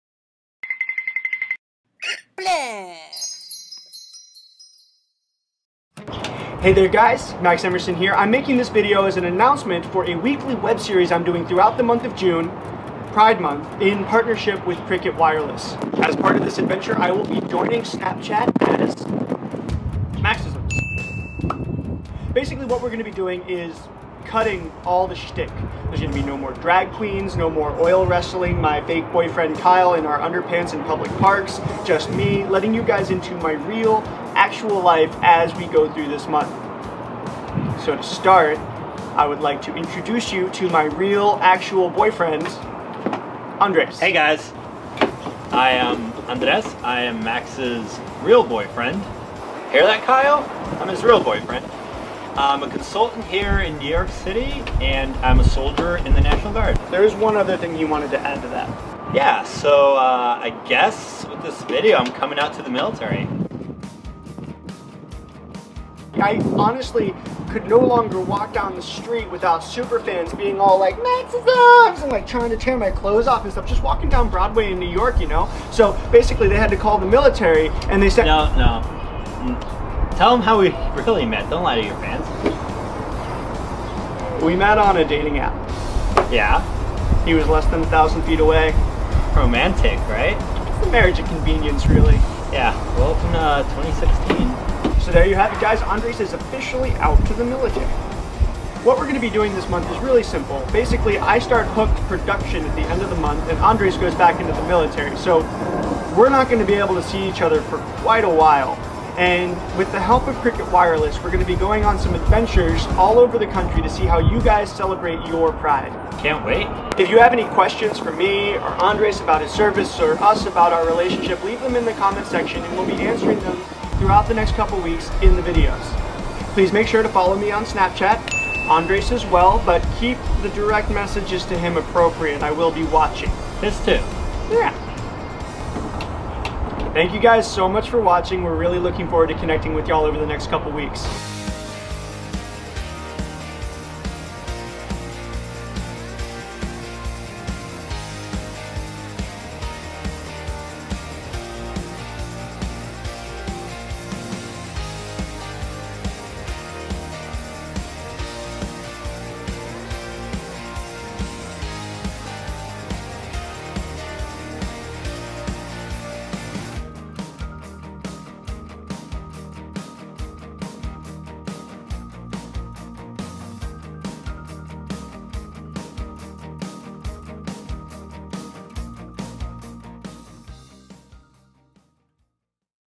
Meet My Boyfriend!
My boyfriend Andres and I will be traveling the country throughout pride month. Please leave any questions for us in the comment section of this video, and we'll answer them in the final episode!